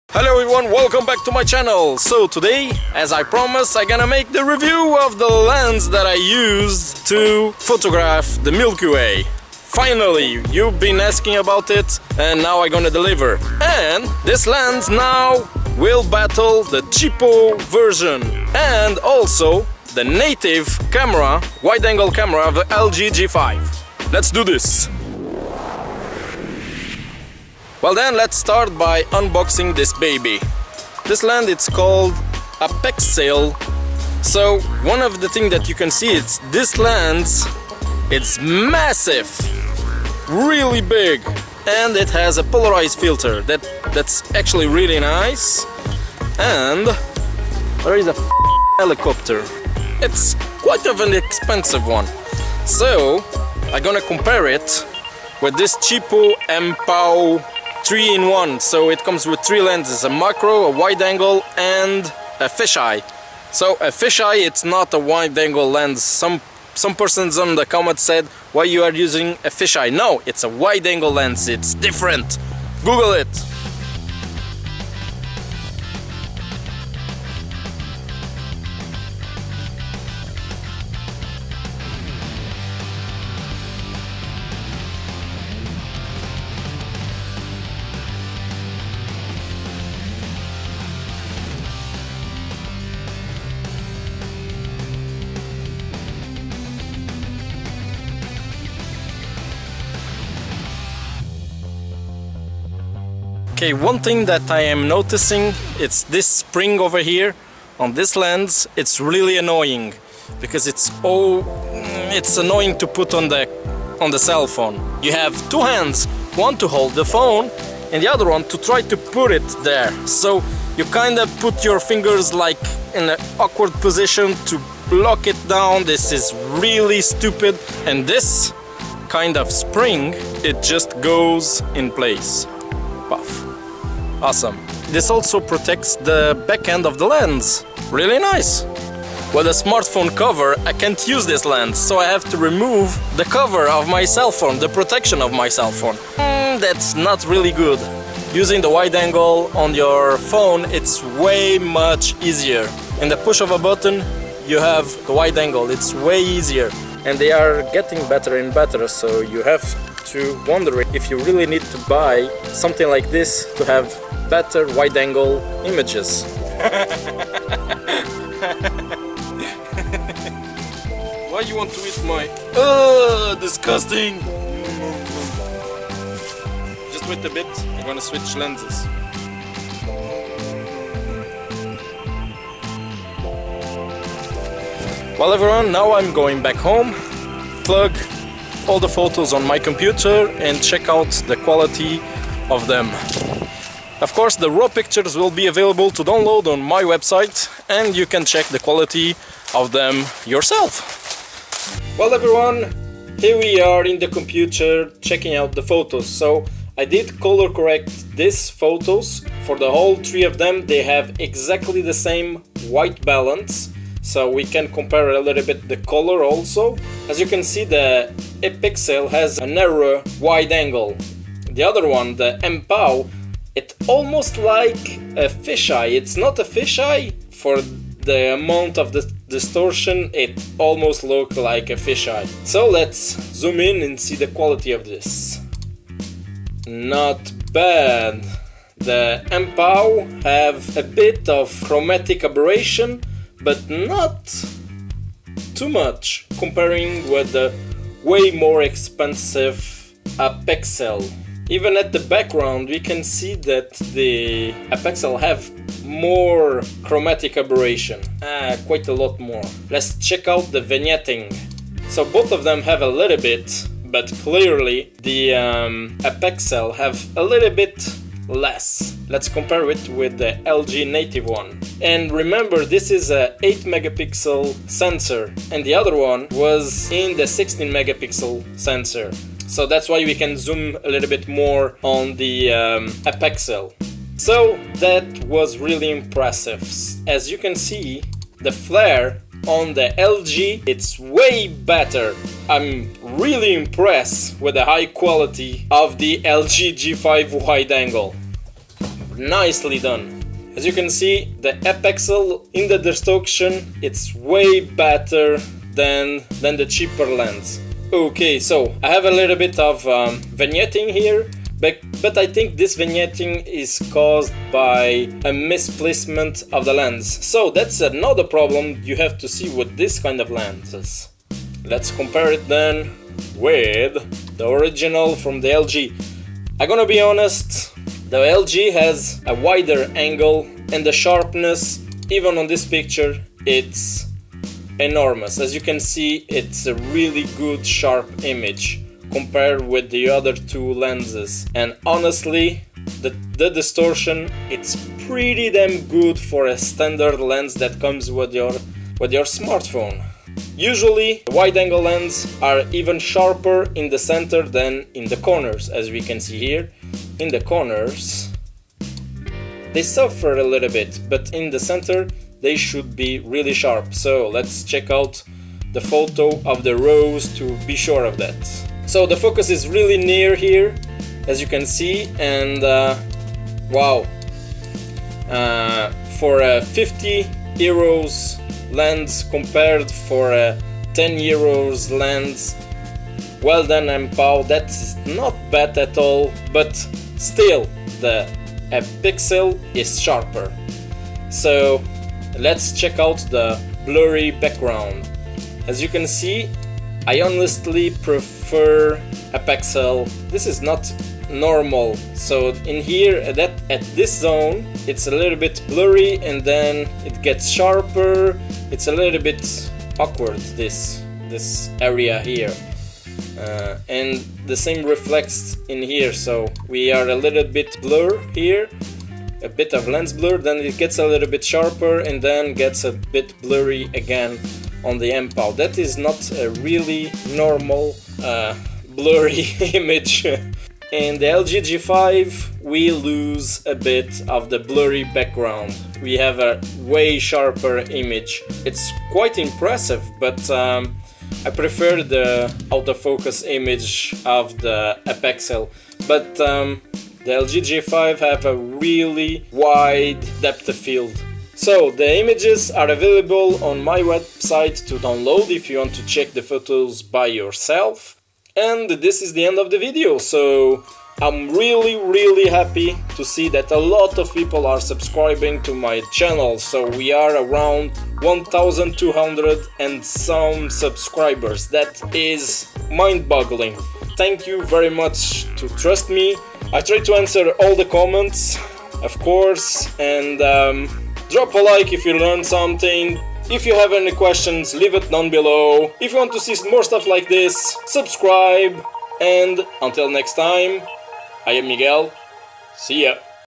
Wide Angle lens for Smartphone Review - Apexel 16mm 4K vs Mpow vs LG G5
After a lot of people ask me about the lens that I used on my tutorial in how to photograph the MilkyWay with a smartphone here is a video review on that lens, the Apexel 16mm 4K and to make it interesting I’m making a comparison with a Mpow that comes on a 10€ kit, and the native one of the LG G5. Just to be clear I’m not a reviewer, so to make it easier I did this comparison.

0:00 Intro: Apexel 16mm 4K VS Mpow VS native wide angle
0:27 Unboxing the Apexel 16mm 4K
1:43 Lock mechanism
2:46 Horsin around...!
3:22 Checking the Chromatic Aberration, Vignetting & Distortion
6:08 Sharpening & out of focus area